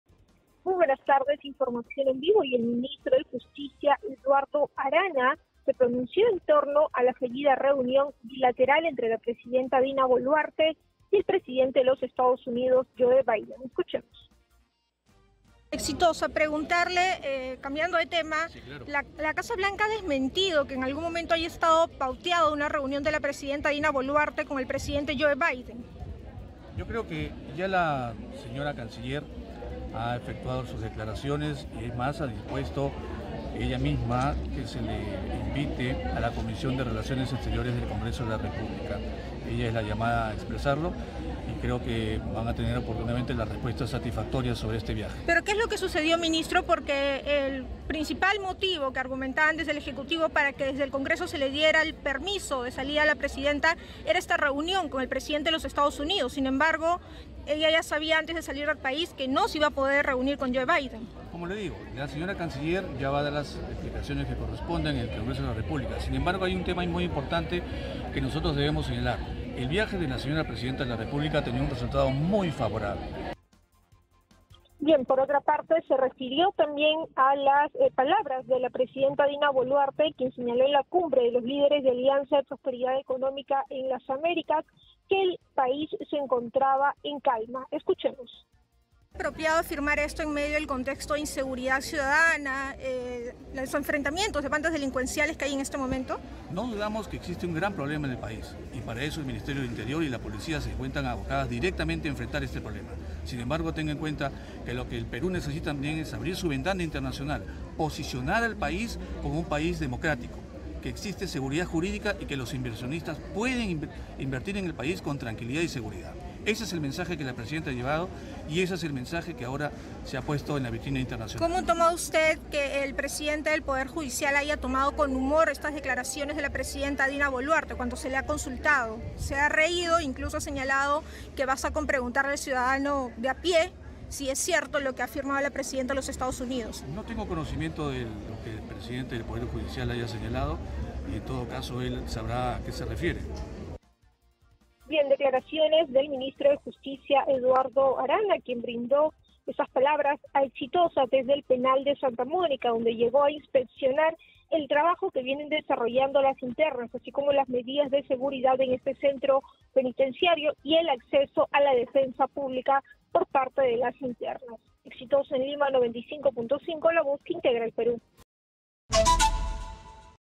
🔴🔵Ministro de Justicia sobre declaraciones de Dina Boluarte en EE.UU.
En declaraciones para Exitosa, el ministro de Justicia, Eduardo Arana, se pronunció sobre declaraciones de la presidenta Dina Boluarte en EE.UU.
"No dudamos que existen problemas en el país", indicó.

Exitosa Perú
"Exitosa: La voz que integra al Perú"

Corporación Universal